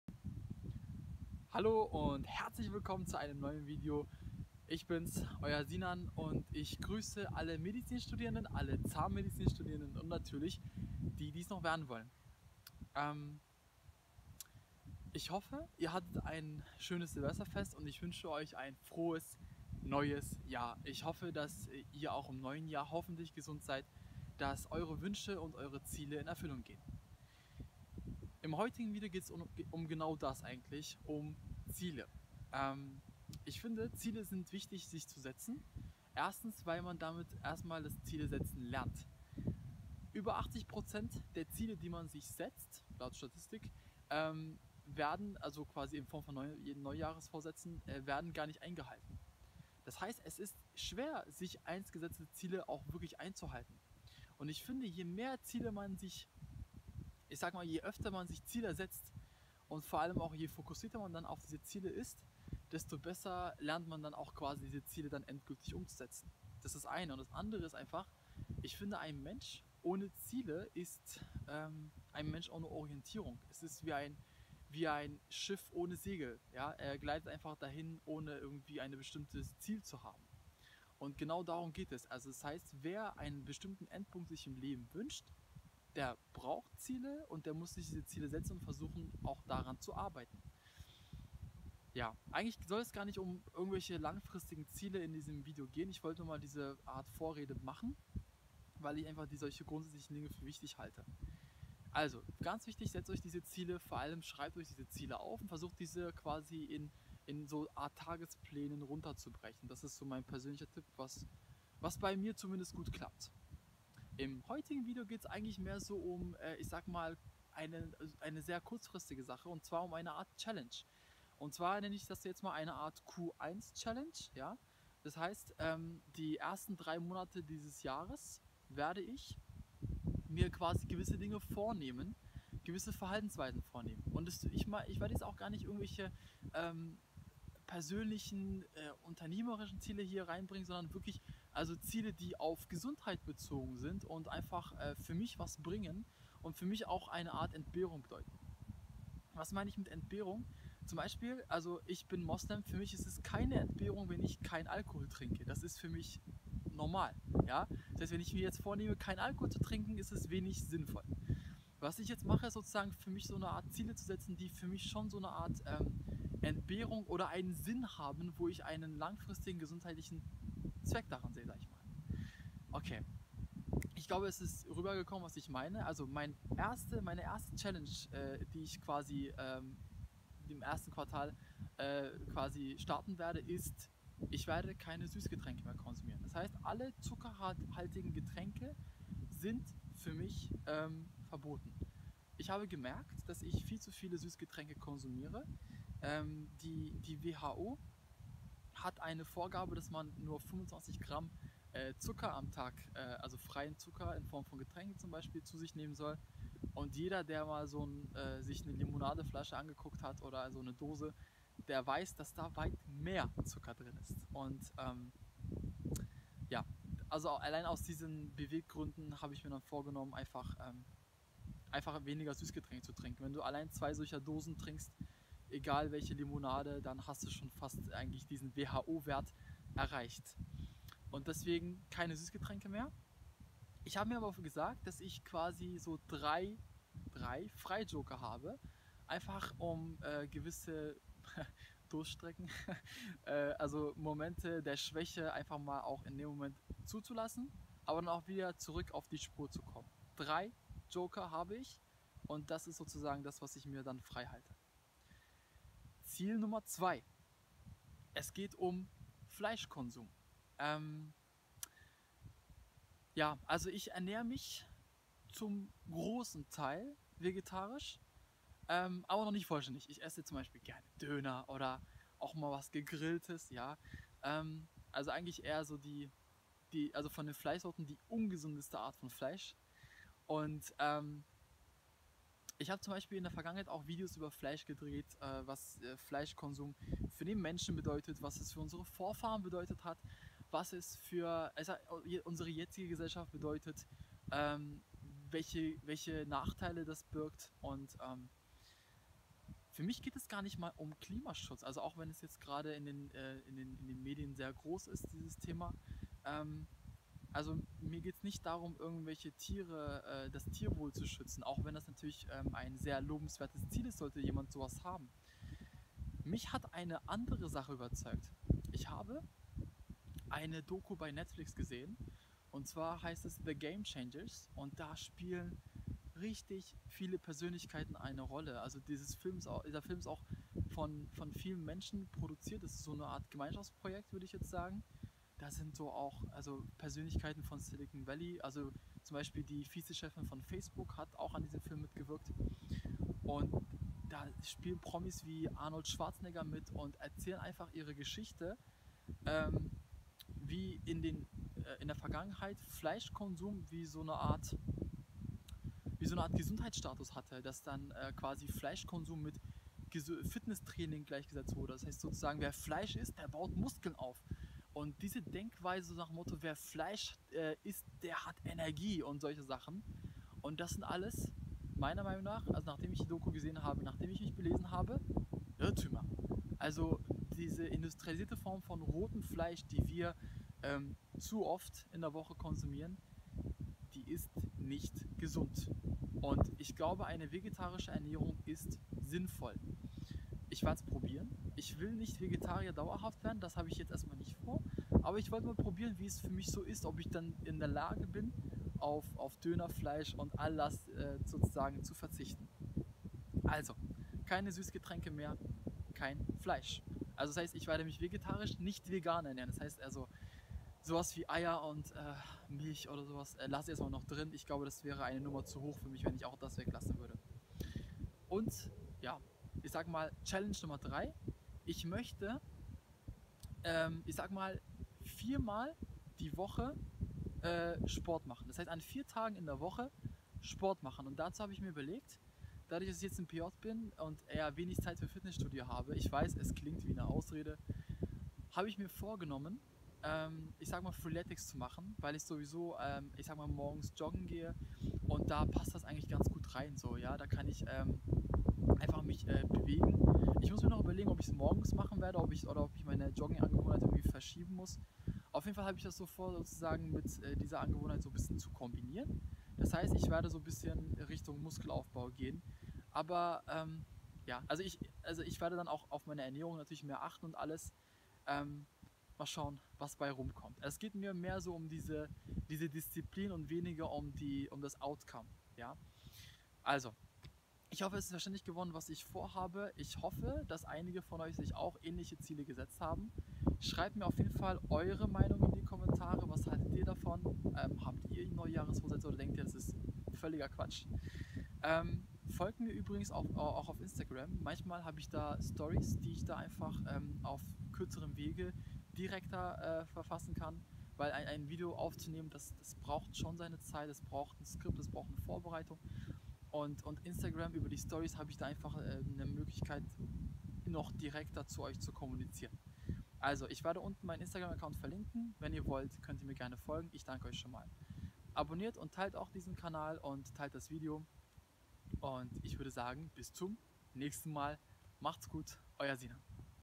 Meine Q1-Challenge - Meine Neujahresvorsätze
Hallo und herzlich willkommen zu einem neuen Video. Heute gehts um meine kurzfristigen Verhaltensziele. Ich wünsche euch viel Spaß.
Euer Sinan 👋